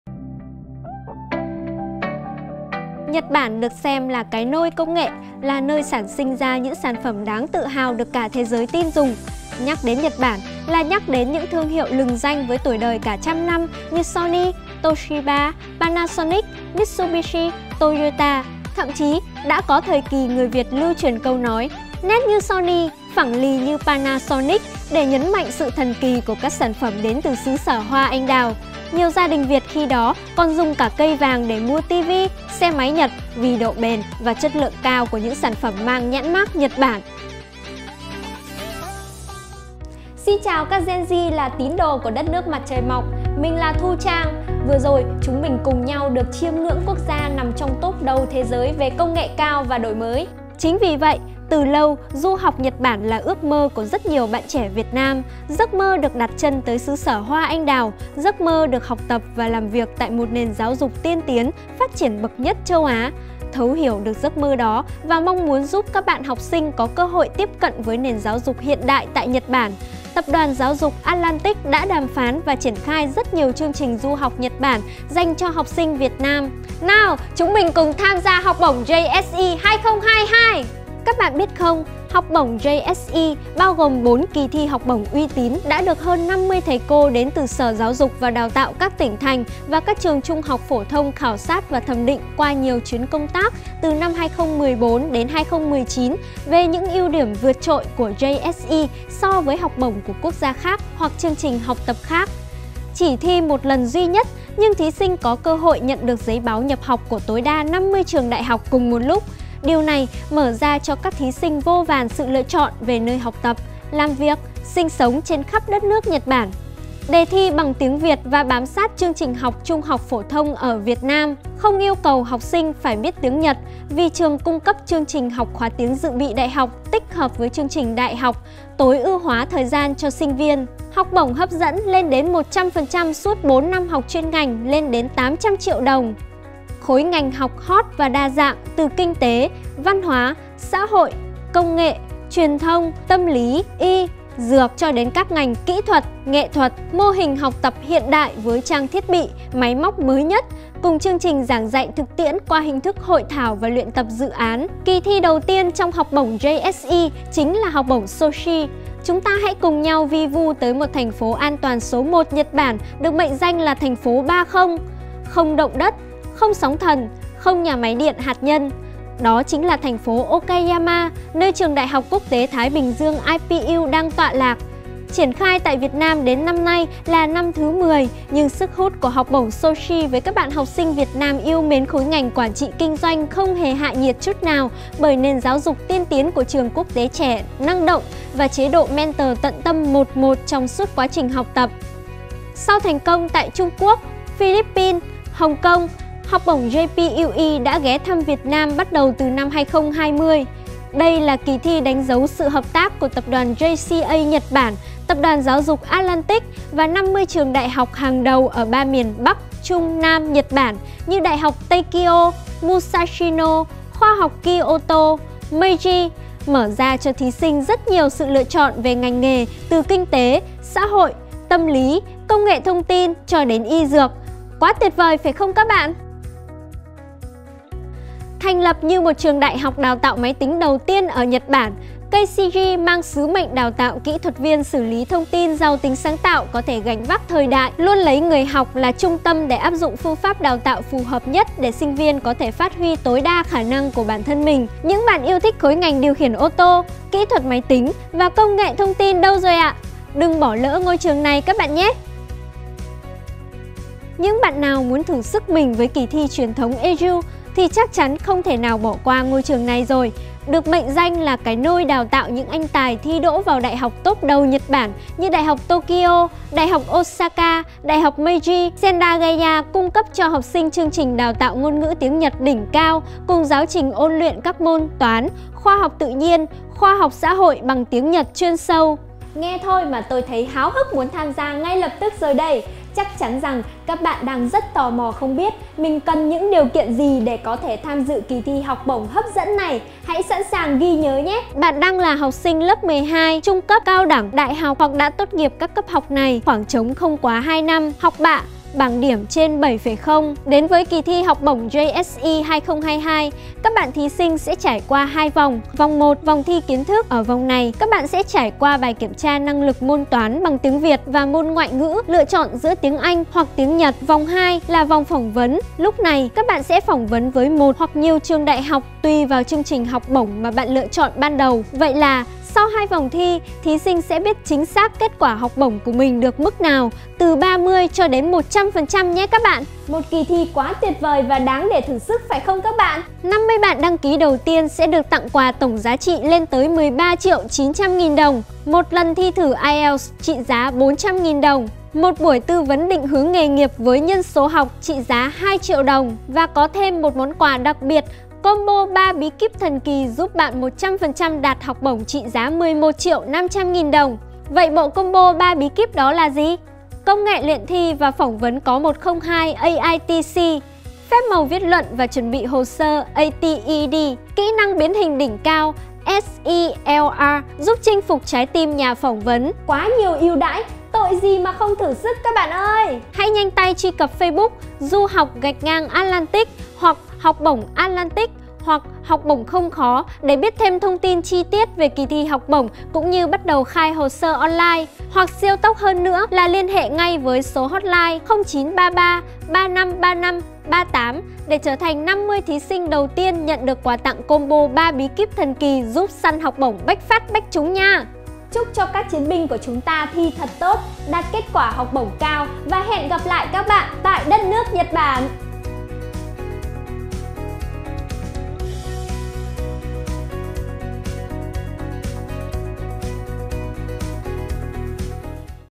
HỌC BỔNG NHẬT BẢN JSE 2022 - ĐẦU VÀO 50 TRƯỜNG ĐẠI HỌC
JSE 2022 – Mở lối thành công  (Japanese Scholarship Examination) là kỳ thi xét tuyển của 4 loại học bổng: Soshi, JPUE, KCG và Sendagaya.  Học sinh  tham dự kỳ thi sẽ trải qua 3 vòng xét tuyển gồm vòng 1: sơ loại hồ sơ, vòng 2: vòng thi, vòng 3: phỏng vấn.

Bạn sẽ có cơ hội nhận học bổng lên tới 100% học phí trong suốt 04 năm chuyên ngành của 50 trường Đại học lớn tại 3 miền Bắc – Trung – Nam của Nhật Bản chỉ với 1 lần nộp hồ sơ.

Đặc biệt, riêng năm 2022 này, 50 bạn đăng ký đầu tiên sẽ có cơ hội nhận quà tặng lên tới 13,9 triệu với bộ combo 3 bí kíp thần kỳ giúp săn học bổng BÁCH PHÁT, BÁCH TRÚNG!

Mọi thông tin chi tiết về kỳ thi KSE Hàn Quốc, các bạn xem tại:
Atlantic Group:

Trụ sở chính:  33 Lạc Trung, Hai Bà Trưng, Hà Nội
Hotline: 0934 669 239/ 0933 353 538 (Ms. Phuong)